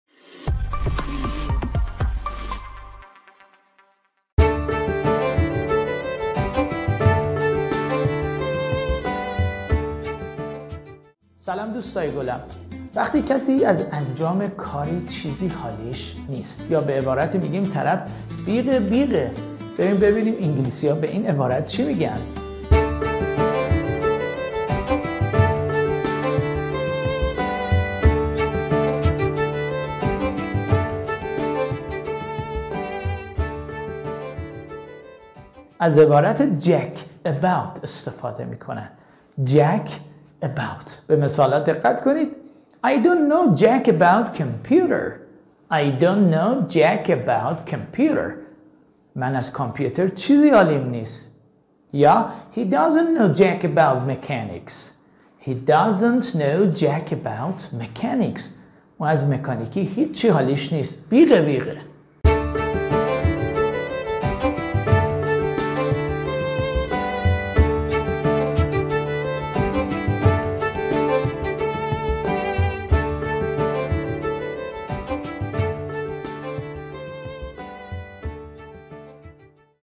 آموزش مکالمه زبان انگلیسی/آموزش اصطلاحات انگلیسی jack about
در این کلیپ اصطلاح jack about  بیق بیق بودن درباره چیزی
به مثال های زیر توجه کنید:
I don't know jack about computer
من اصلا درباره کامپیوتر چیزی نمیدانم
He doesn't know jack about mechanics.
او از مکانیکی هیچی حالیش نیست.